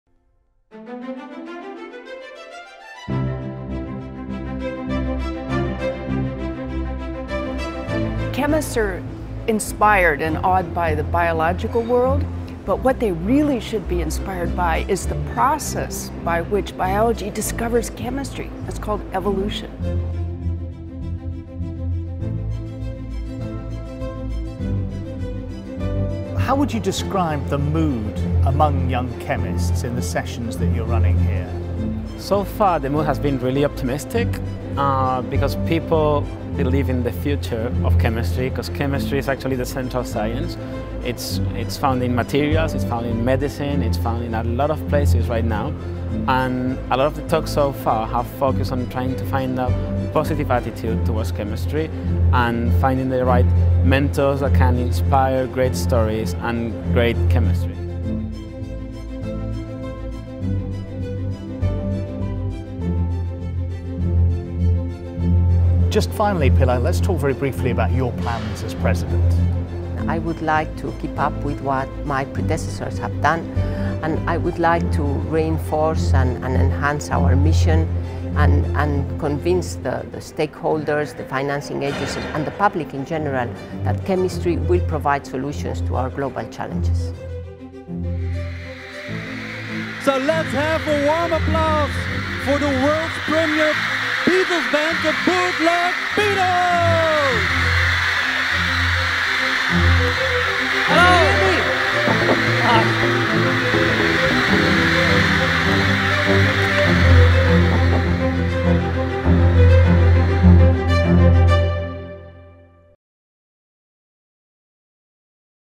Highlights from the second day of EuChemS2018
Highlights from the second day of the 7th EuCheMS Chemistry Congress included plenary lectures from Professors Omar Yaghi and Frances Arnold. We also spoke to representatives of the European Young Chemists Network and EuChemS president, Pilar Goya Laza.

You can also see their full interviews on this channel and you can follow all the action from the congress on twitter and facebook EuChems2018.